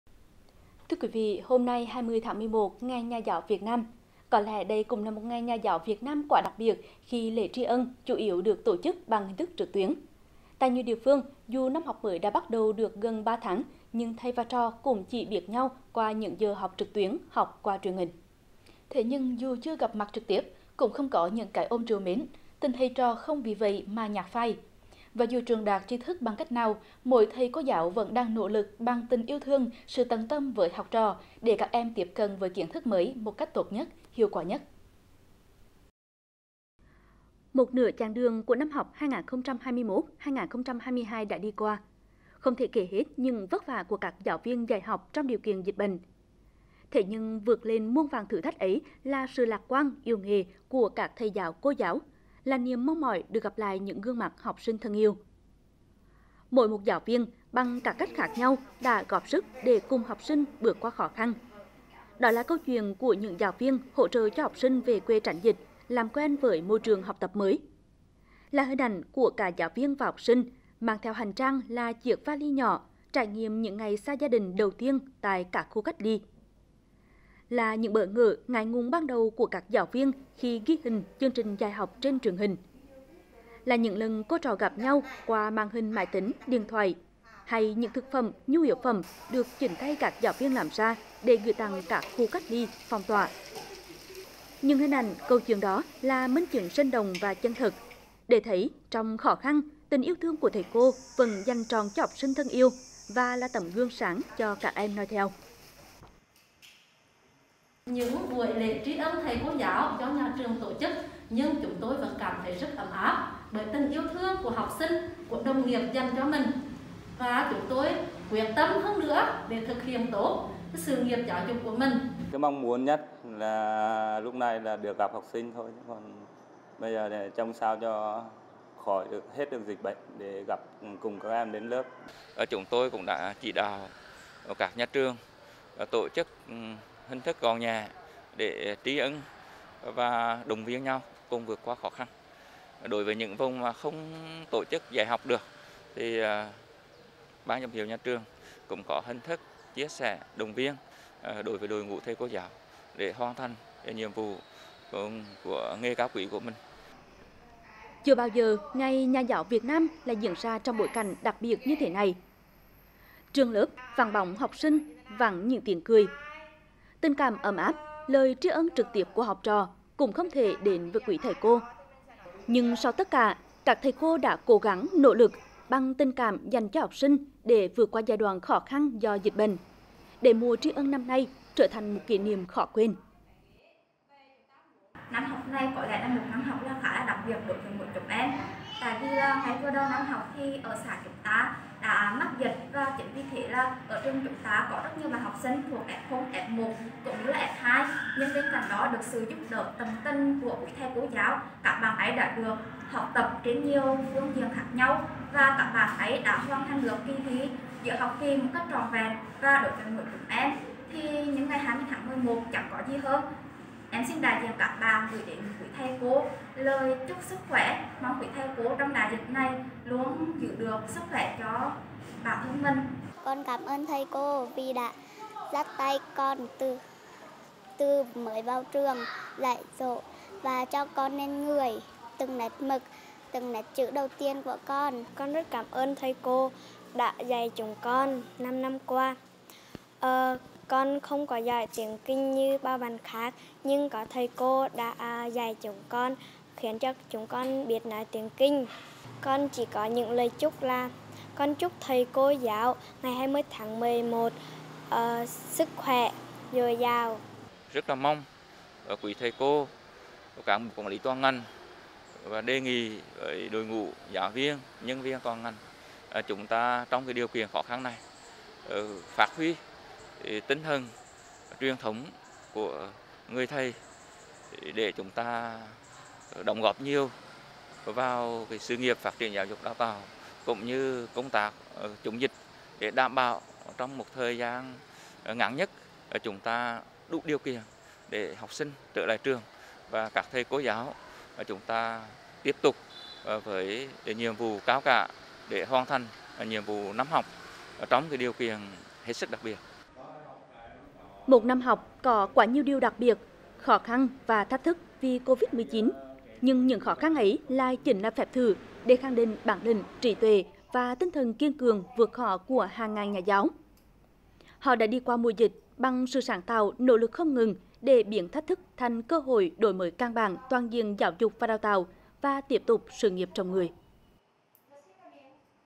Mùa tri ân đặc biệt
Hôm nay 20/11, ngày Nhà giáo Việt Nam.  Có lẽ đây cũng là một ngày Nhà giáo Việt Nam quá đặc biệt khi lễ tri ân chủ yếu được tổ chức bằng hình thức trực tuyến. Tại nhiều địa phương, dù năm học mới đã bắt đầu được gần cả ba tháng nhưng thầy và trò cũng chỉ biết nhau qua những giờ học trực tuyến, học qua truyền hình. Thế nhưng, dù chưa gặp mặt trực tiếp, cũng không có những cái ôm trìu mến, tình thầy trò không vì vậy mà nhạt phai. Và dù truyền đạt tri thức bằng cách  nào, mỗi thầy cô giáo vẫn đang nỗ lực bằng tình yêu thương, sự  tận tâm với học trò để các em tiếp cận kiến thức mới một cách tốt nhất, hiệu quả nhất.